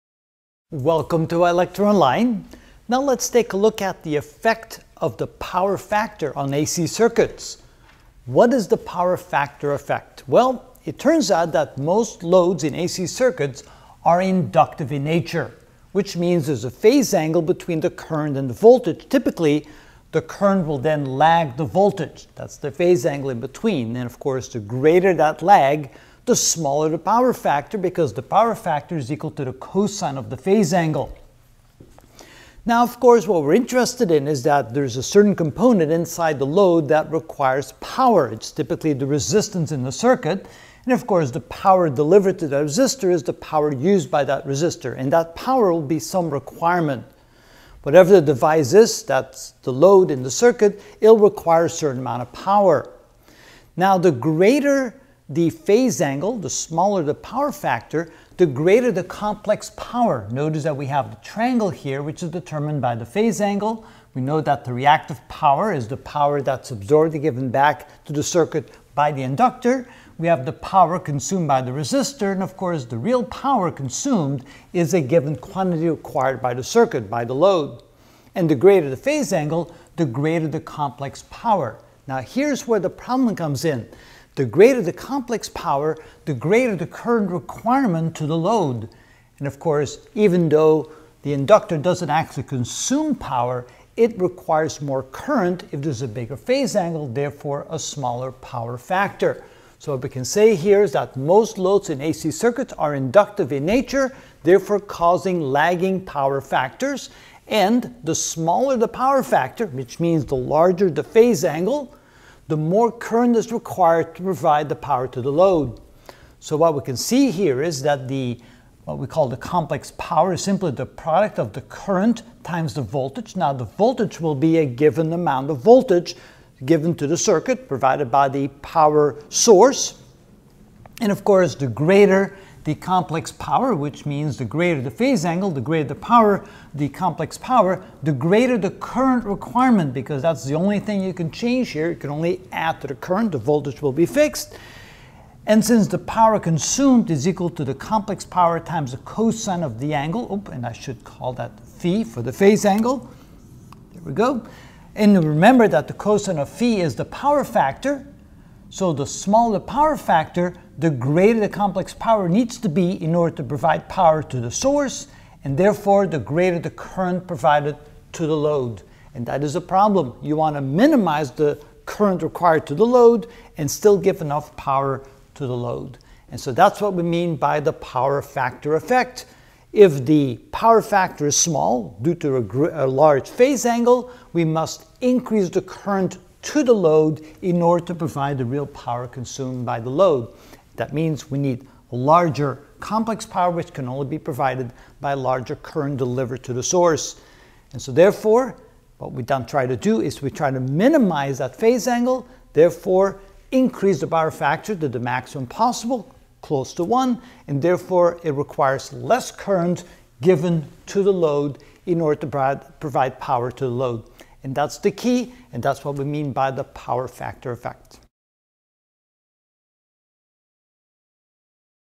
Electrical Engineering: Ch 12 AC Power (47 of 58) What is the Power Factor Effect?
In this video I will explain the effect of the power-factor of an AC circuit. It turns out that most loads in an AC circuit is inductive in nature. Which means there's a phase angle between the current and voltage. Typically the current will lag the voltage. The greater the lag the smaller power-factor, because the power-factor is the cosine of the angle.